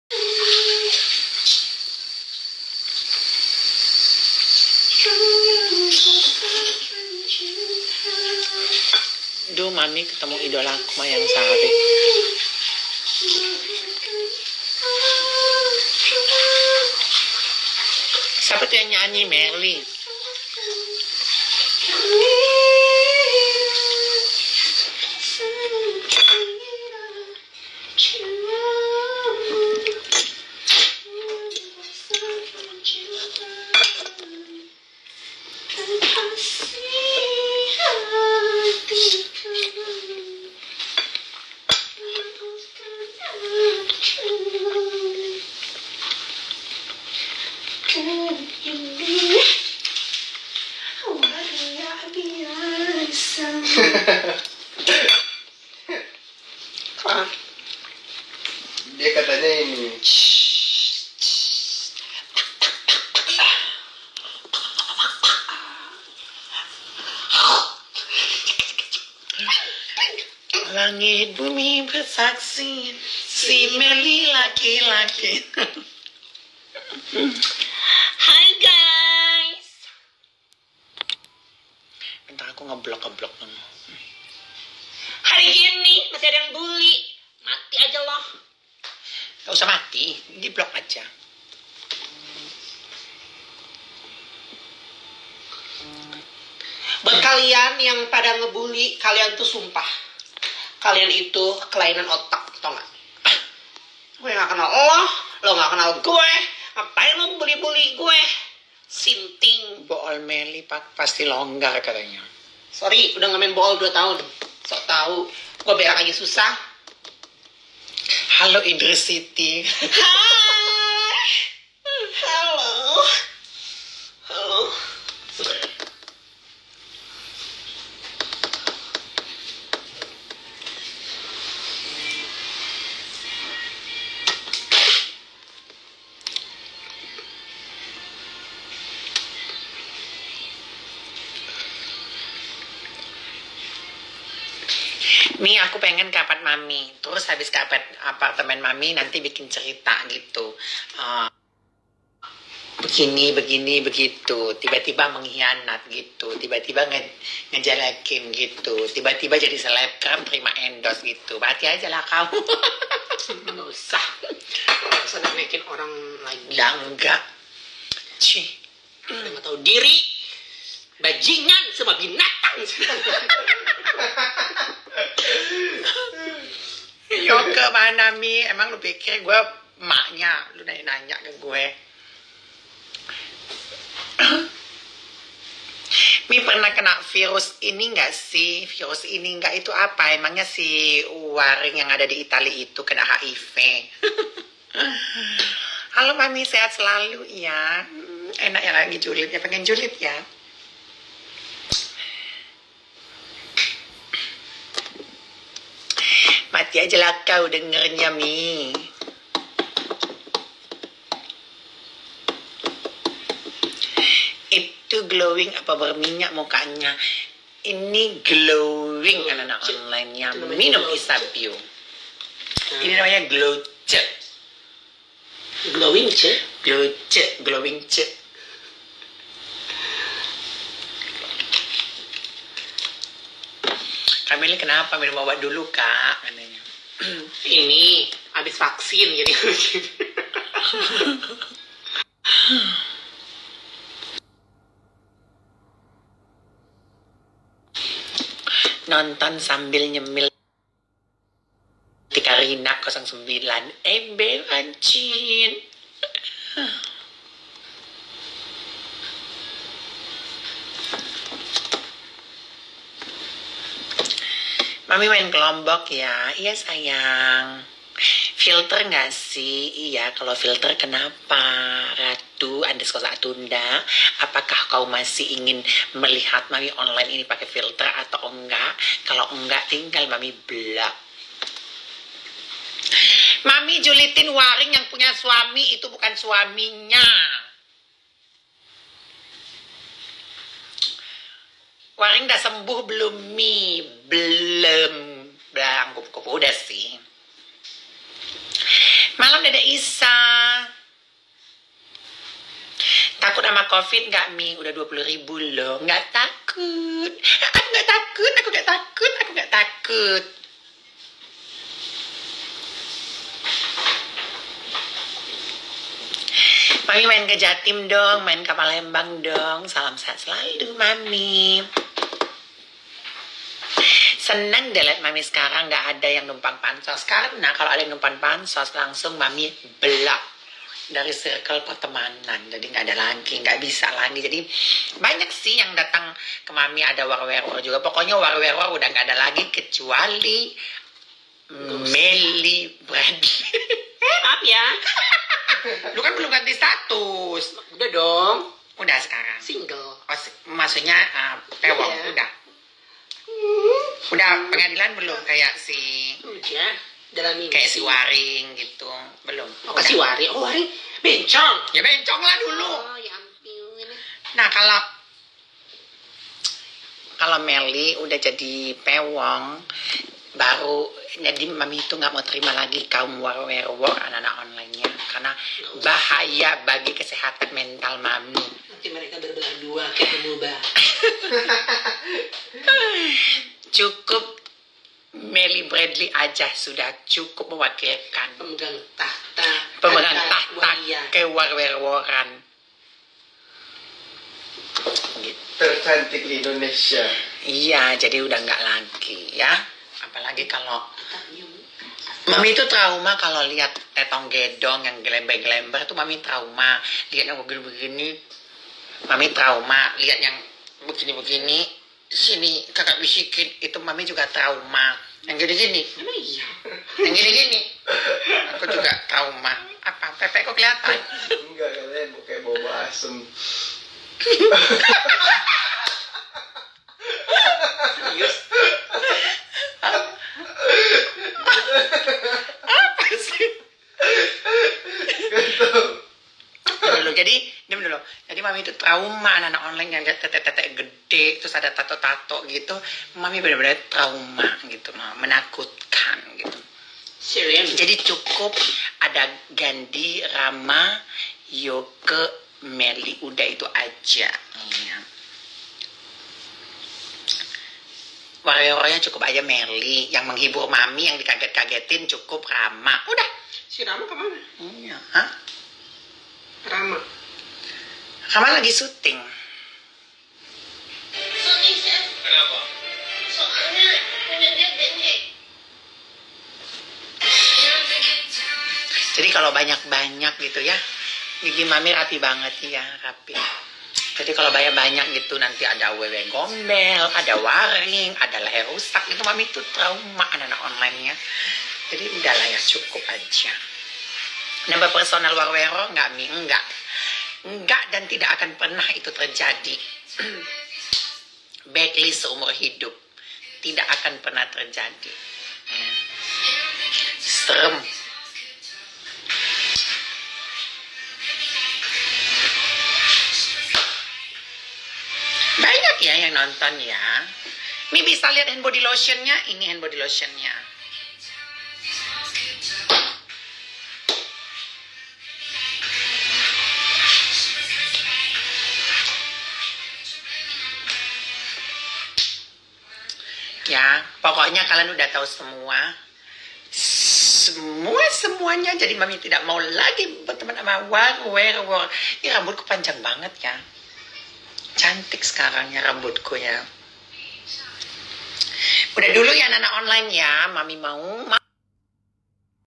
ISA ZEGA & MELLY BRADLEY peringatkan HATERS untuk stop BULLY mereka atau akan berurusan dengan HUKUM
Silahkan komen dikolom komentar apabila ada pertanyaan yg ingin ditanyakan.

Terima Kasih 😊